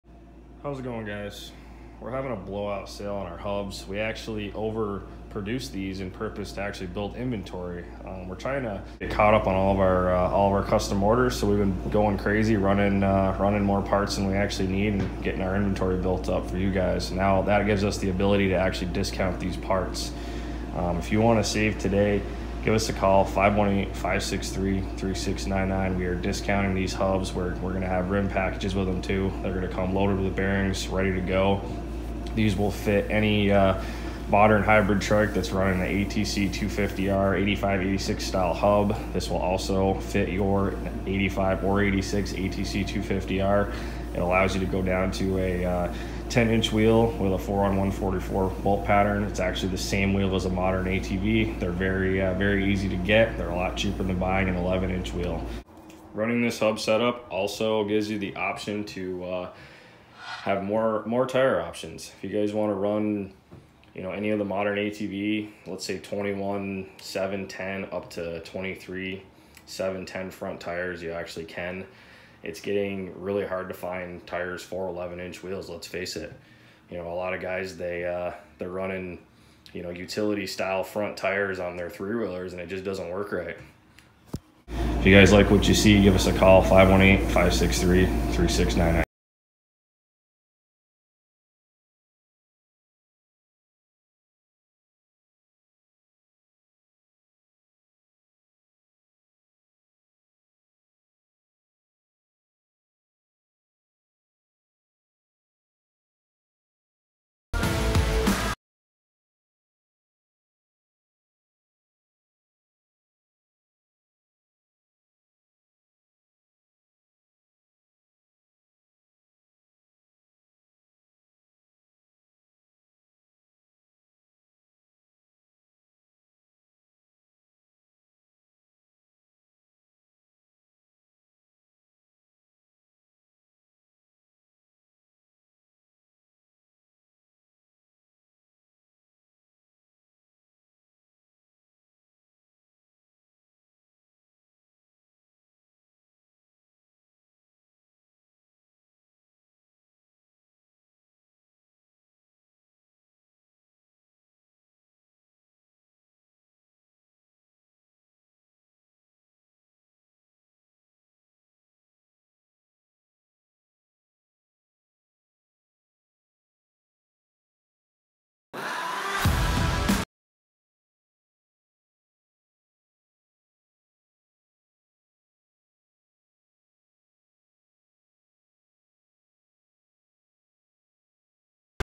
Honda ATC250R Billet front hub?! More Tire options? BVC Trikes 10” hub & Wheel Setup
Do you own an 85-86 Honda ATC250R or a modern trike that uses the same hub?
We now offer a billet hub that allows the use of a modern 10” atv wheel. This also gives you unlimited tire options.

No more searching for tires that fit that 11” wheel!